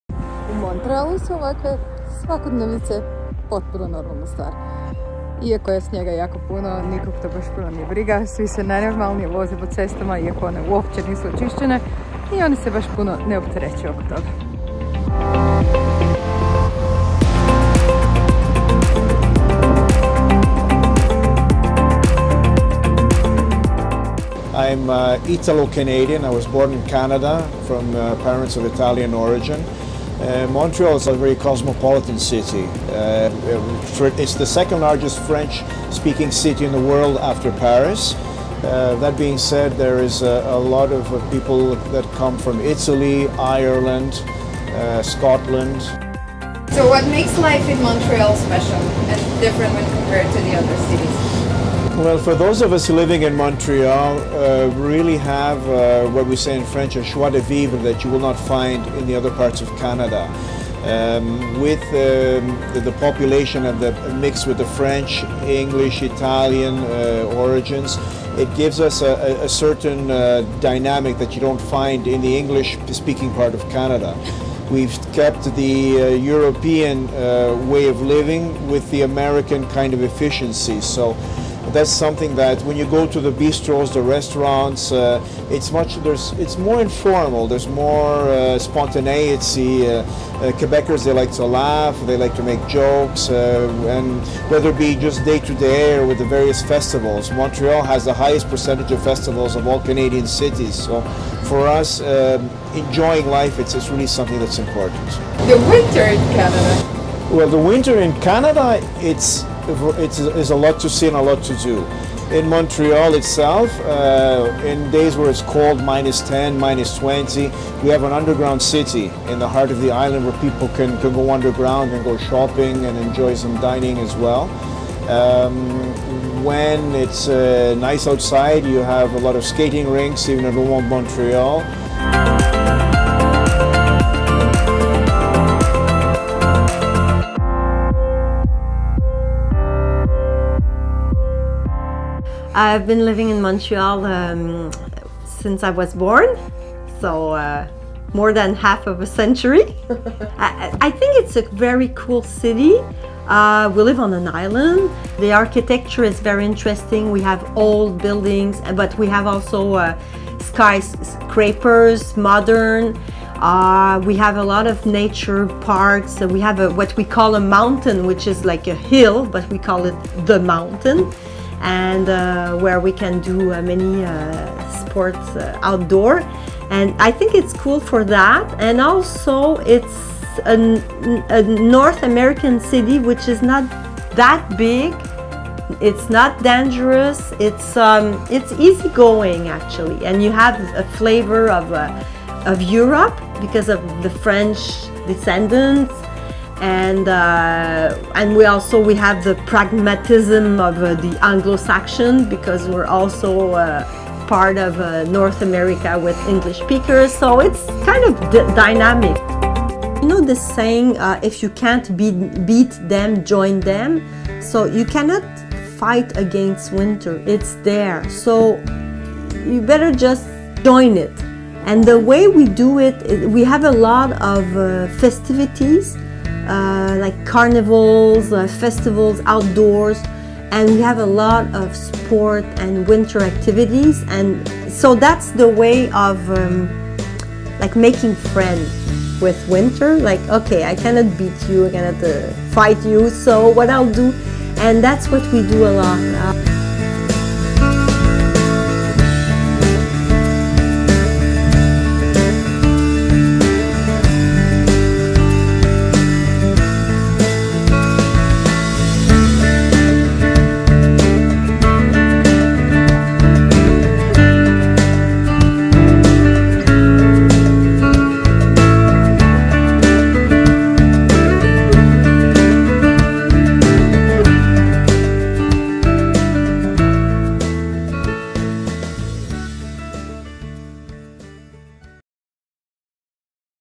Montreal and Laval, Quebec, Canada
Get to know what so cool about these two Canadian islands- cities... talking to locals and what they have to say abut it ;)